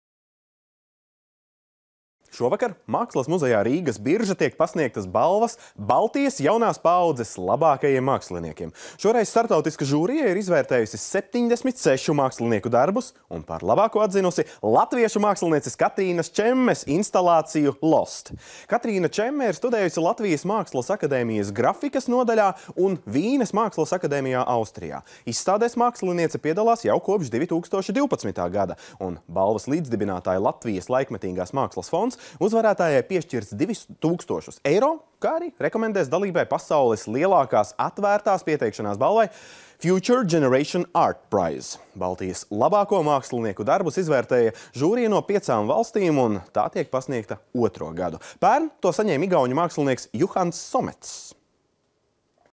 Baltijas jaunās paaudzes mākslinieku balva
Šovakar Mākslas muzejā Rīgas Birža tiek pasniegtas balvas Baltijas jaunās paaudzes labākajiem māksliniekiem. Šoreiz starptautiskā žūrija izvērtēja 76 mākslinieku darbus. Un par labāko atzina latviešu mākslinieces Katrīnas Čemmes instalāciju “Lost”.